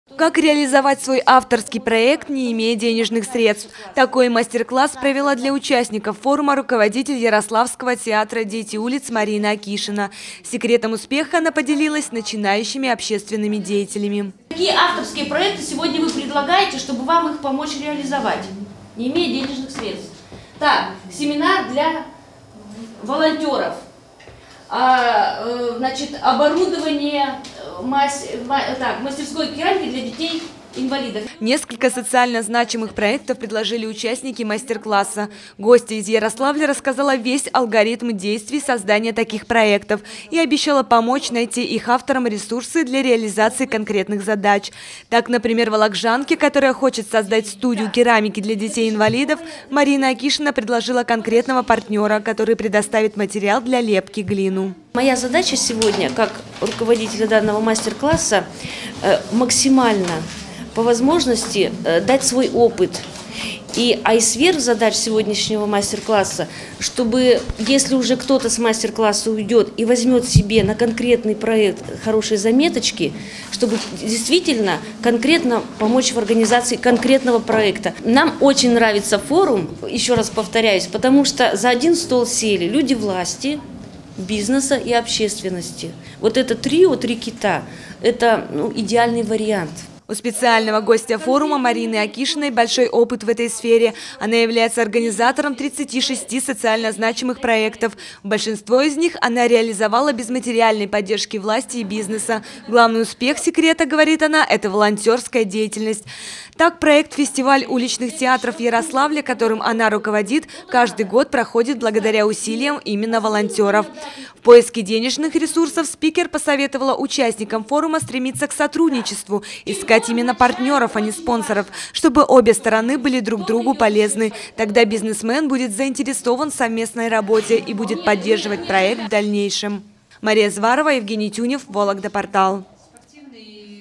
Руководитель театра «Дети улиц» Марина Акишина поможет вологжанам реализовать свои авторские проекты
Гостья из Ярославля провела для участников форума мастер-класс «Как реализовать свой авторский проект». Она подробно рассказала весь алгоритм действий и пообещала помочь начинающим общественным деятелям найти некоторые ресурсы для реализации конкретных задач.